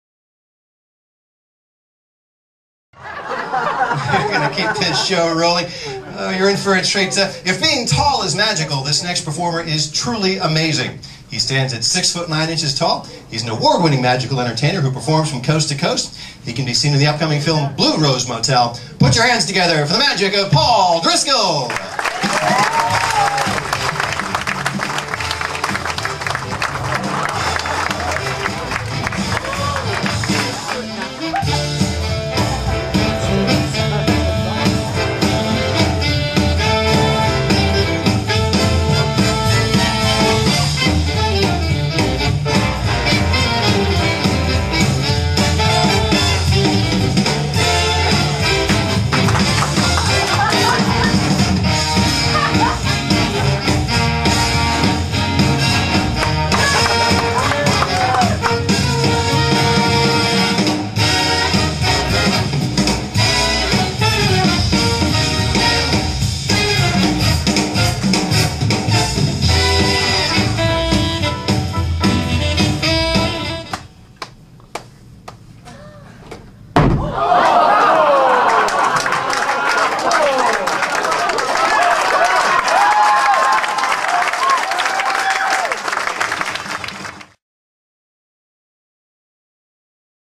Magician PAUL DRISCOLL - Bowling Ball magic
Award winning magician Paul Driscoll performs a jaw dropping opening trick from his headlining act at the famed Magic Castle night club in Hollywood.